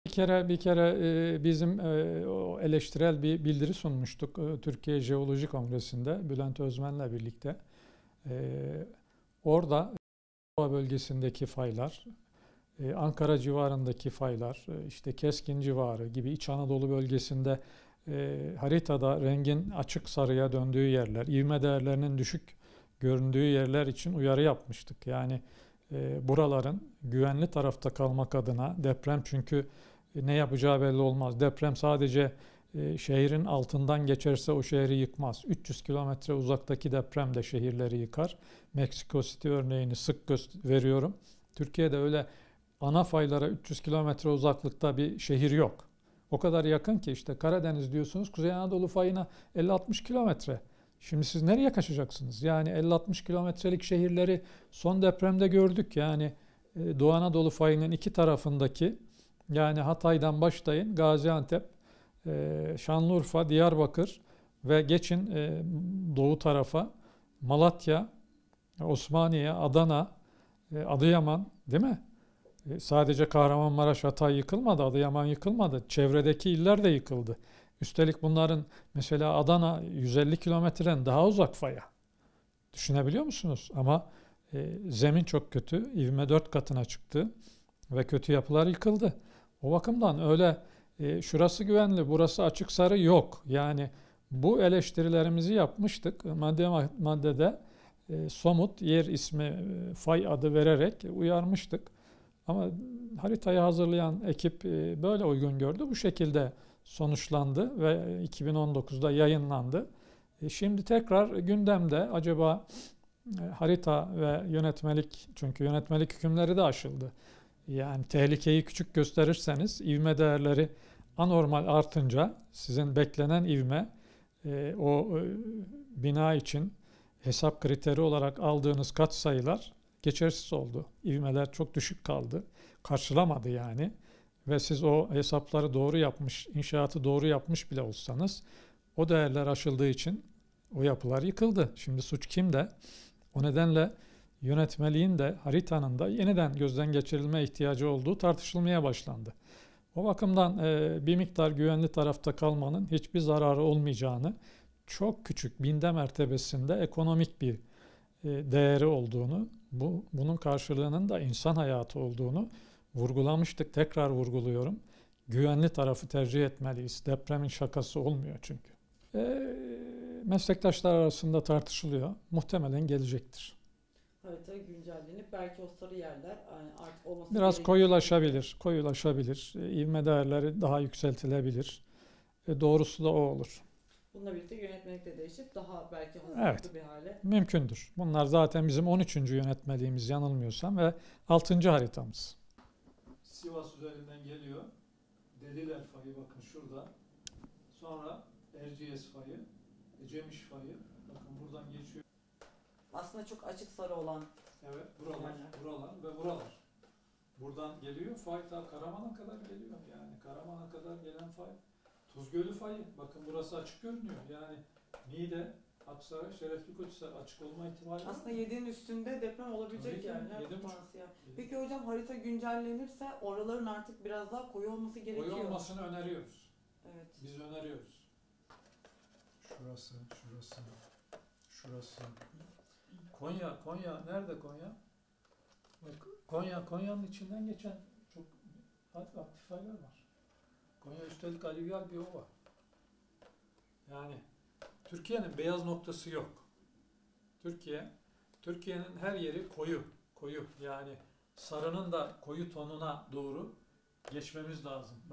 Prof. Dr. Pampal: 'Türkiye Deprem Tehlike Haritası' Güncellenmeli
Gazi Üniversitesi Deprem Araştırma Merkezi Kurucu Başkanı Prof. Dr. Süleyman Pampal, AFAD koordinasyonunda hazırlanarak, 1 Ocak 2019 tarihinde yürürlüğe giren 'Türkiye Deprem Tehlike Haritası'nın güncellenmesi gerektiğini söyledi. Pampal, harita henüz taslak aşamasındayken Çukurova bölgesi, Ankara, Keskin civarı gibi Anadolu bölgesinde rengin açık sarıya döndüğünü, ivme değerleri düşük görülen yerler için o dönem uyarı yaptıklarını belirtip, "Muhtemelen harita güncellenecek. Sarı yerler koyulaşabilir, ivme değerleri daha yükseltilebilir, doğrusu bu olur" dedi.
Şaduman TÜRKAY- Canberk ÖZTÜRK/ ANKARA, (DHA)-